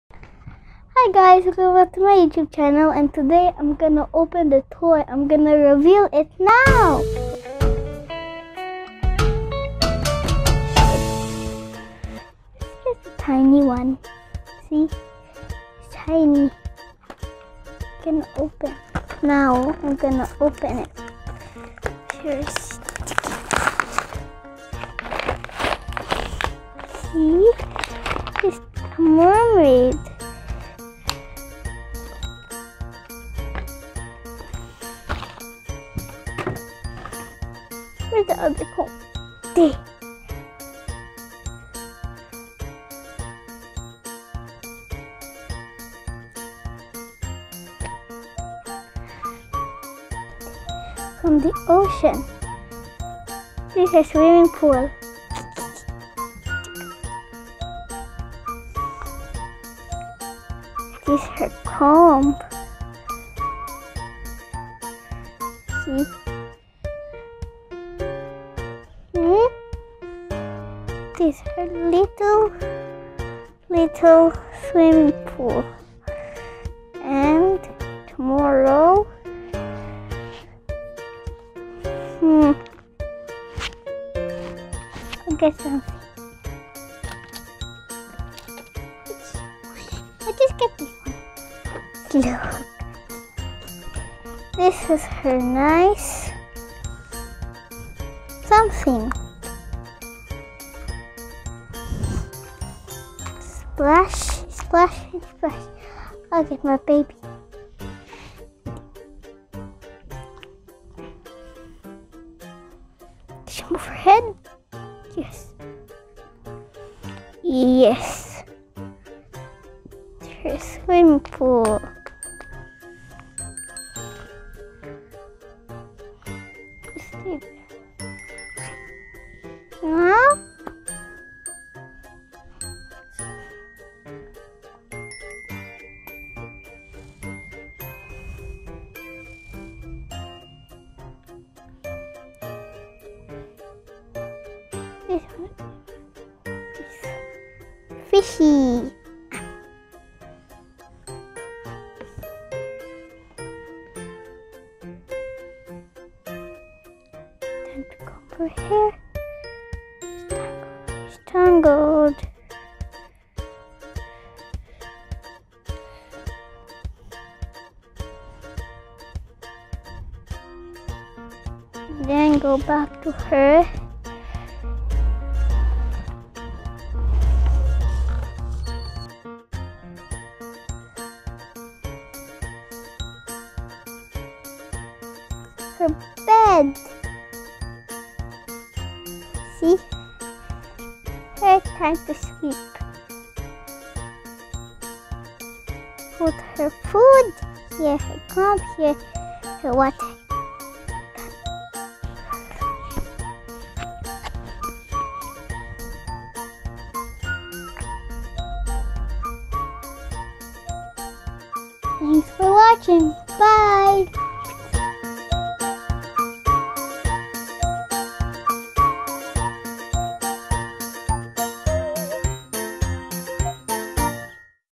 Rania is playing her toy a Little Mermaid
Rania is unwrapping and  playing her new toy a tiny "Little Mermaid"